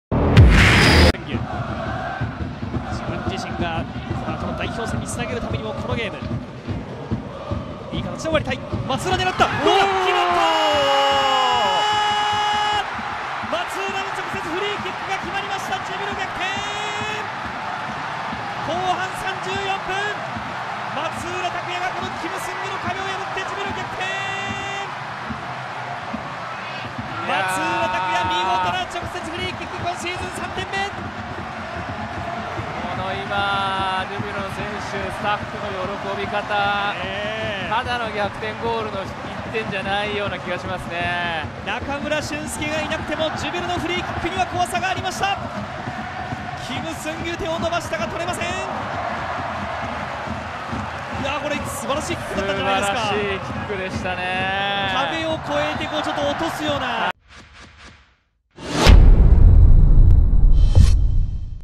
【公式】ゴール動画：松浦 拓弥（磐田）79分 ジュビロ磐田vsヴィッセル神戸 明治安田生命Ｊ１リーグ 第24節 2017/8/26
松浦 拓弥（磐田）が直接FKをゴール右上に蹴り込んで勝ち越しゴールを奪う！
2017年8月26日（土）に行われた明治安田生命Ｊ１リーグ 第24節 磐田vs神戸のゴール動画です。